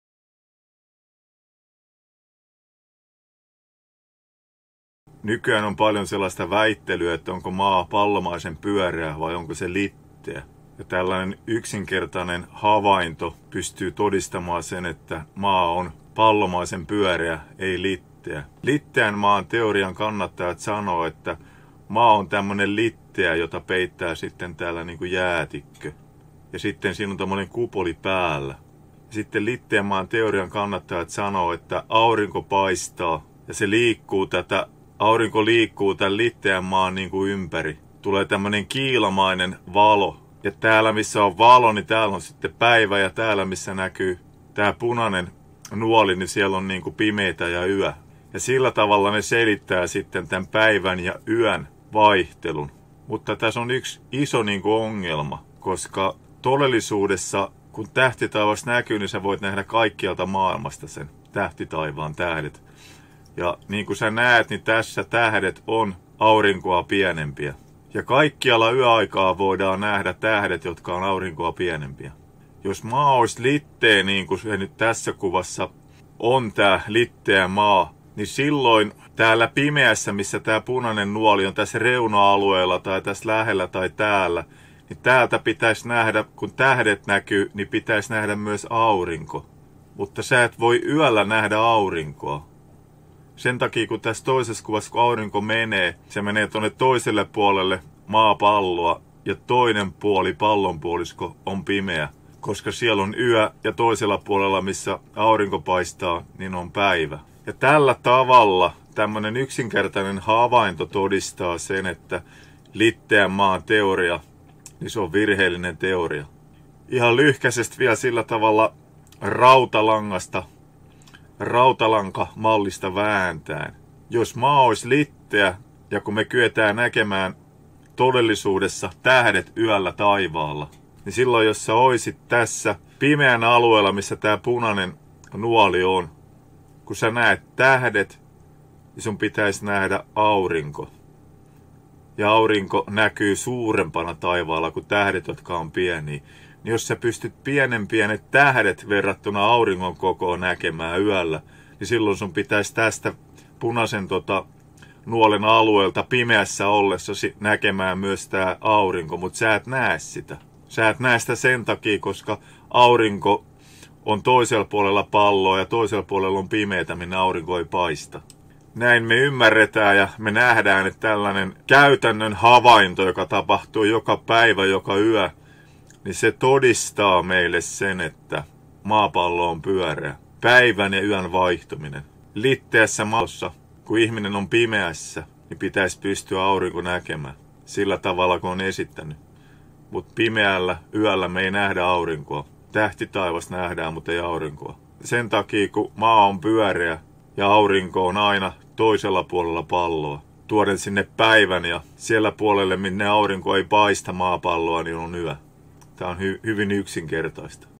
Flat Earth Theory Disproved
Flat Earth Theory Disproved. Audio in Finnish and English Subtitles.